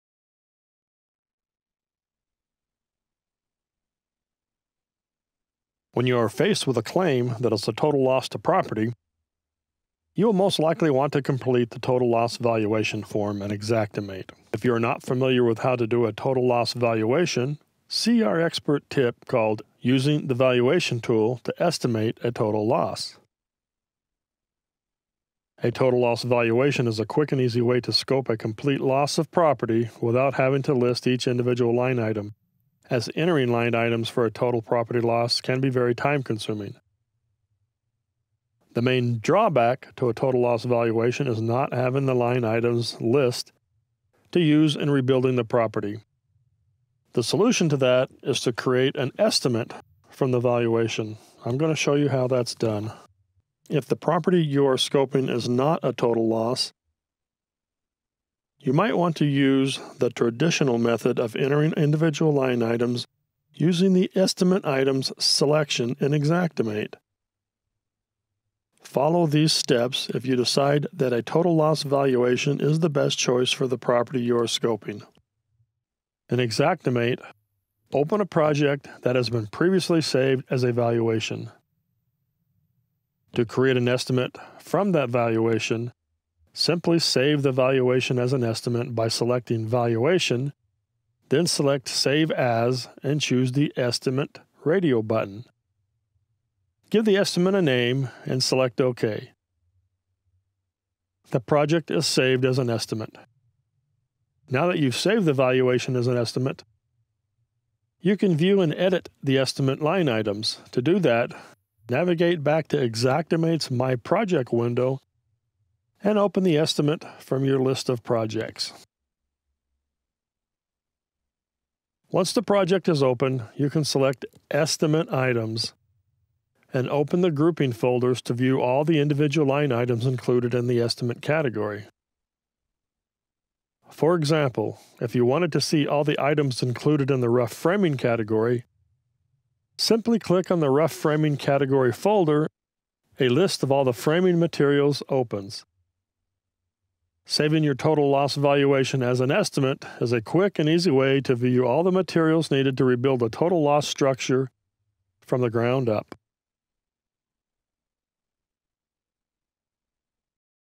Xactimate Xpert Tip: Create an Estimate from a Valuation in Xactimate
Learn how to create an estimate from a valuation in Xactimate. Desktop Version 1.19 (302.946).

xactimate28,xactimate,xactware,xpert tips,how to,construction,estimating,estimate,estimate items,sketch,adjuster,claims,valuation,total loss